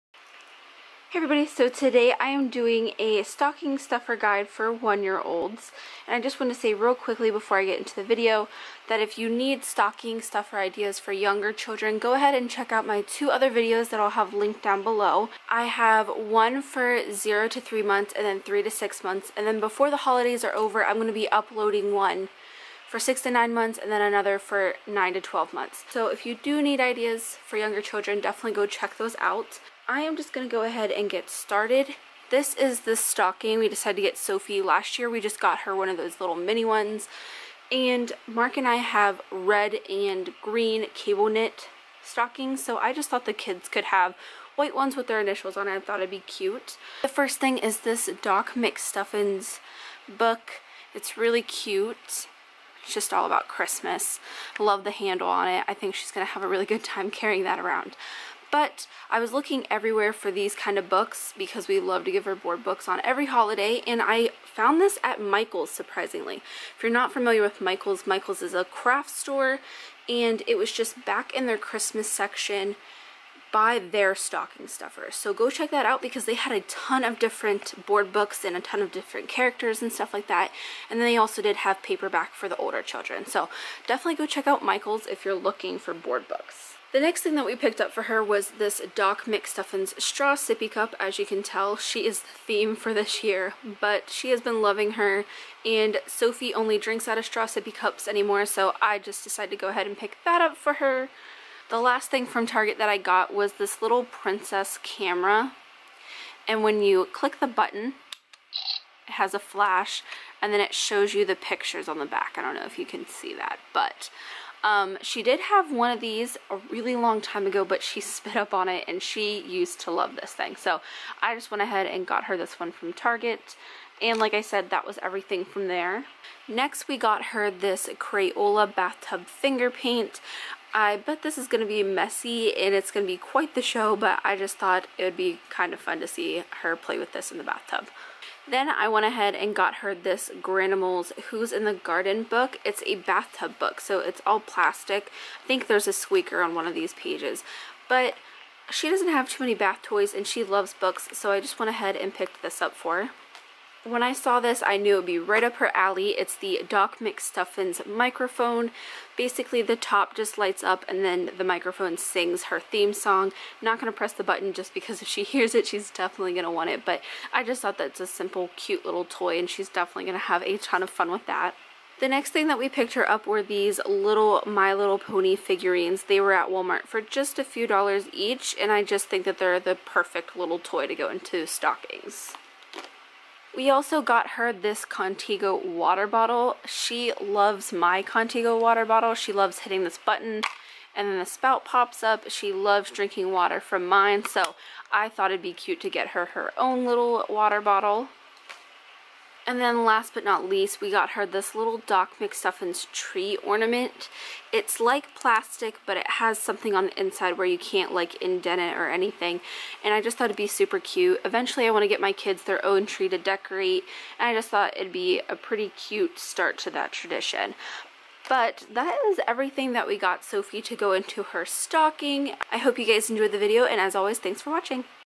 Stocking Stuffer Ideas for 1 Year Olds // Momma Alia
Stocking Stuffer Ideas for 1 Year Olds // Momma Alia. My best stocking stuffer ideas. Please leave any questions or requests you may have!

*I was not sponsored to make this video or by any company I mentioned & all opinions are 110% my own*